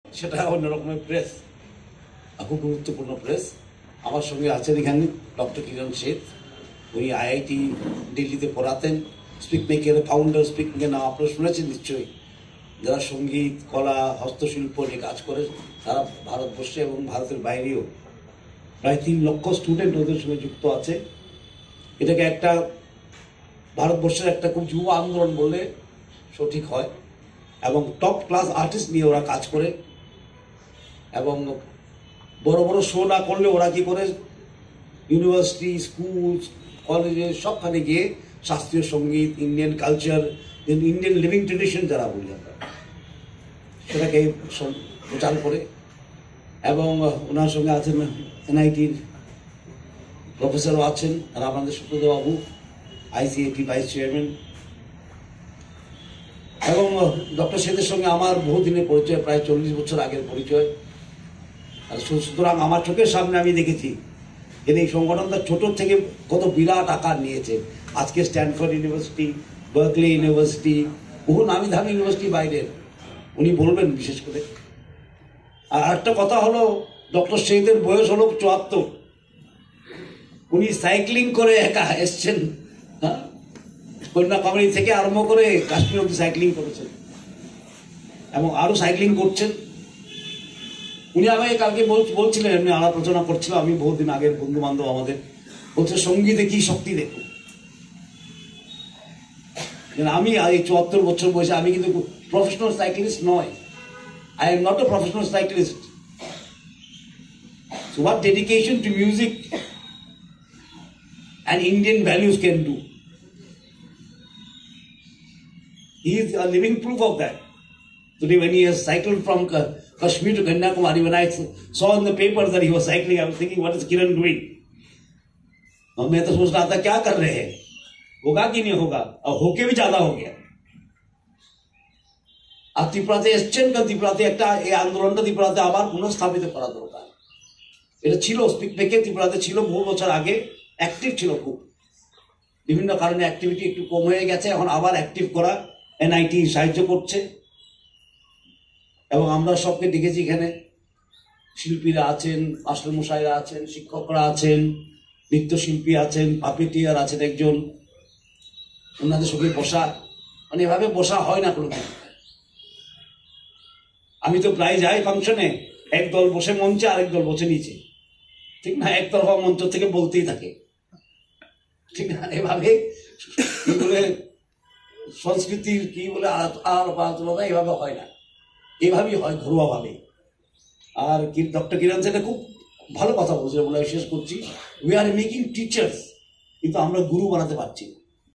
কিরণ শেঠ বুধবার প্রাক্তন উপমুখ্যমন্ত্রী যীষ্ণু দেববর্মণের সঙ্গে সৌজন্যতামূলক সাক্ষাৎ করেন।
স্পিকমিকে প্রতিষ্ঠাতা কিরণ শেঠ বুধবার প্রাক্তন উপমুখ্যমন্ত্রী যীষ্ণু দেববর্মণের সঙ্গে এস পি মুখার্জি লেন স্থিত আবাসনে সৌজন্যতামূলক সাক্ষাৎ করেন।